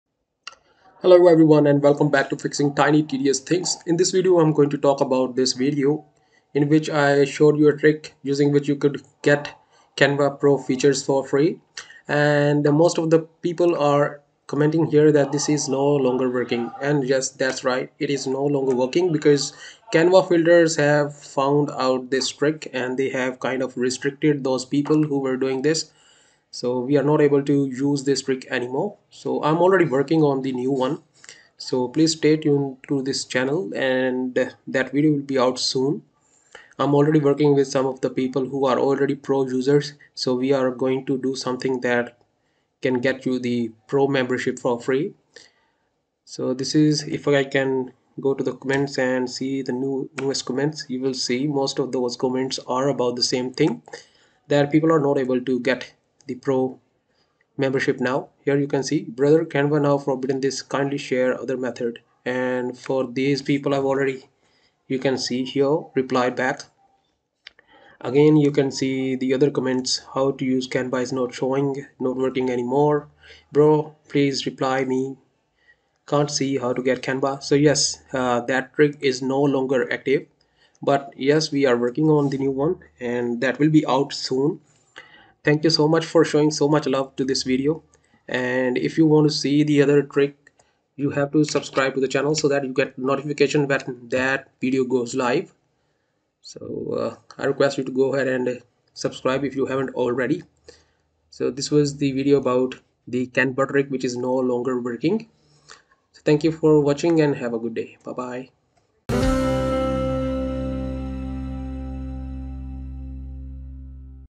Trick to Get Canva Pro For Free No Longer Works - New Trick Soon
Canva Pro Membership for free through Kenba trick is no longer working. I am already working on a new trick and it will be out soon..